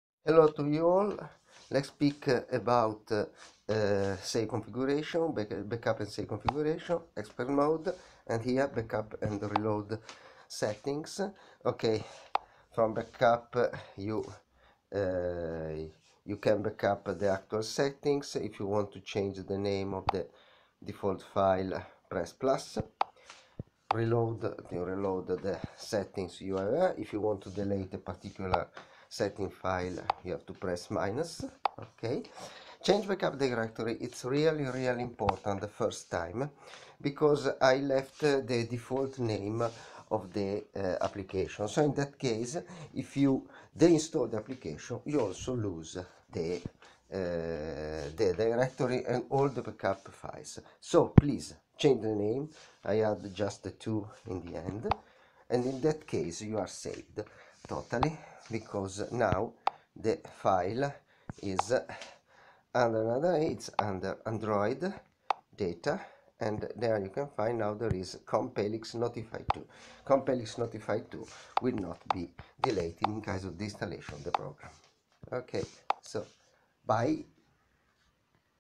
3b1. SETTINGS - BACKUP AND RESTORE
Backup and Restore of the Settings, the forst time I recommend you to change the default name of the Backup directory!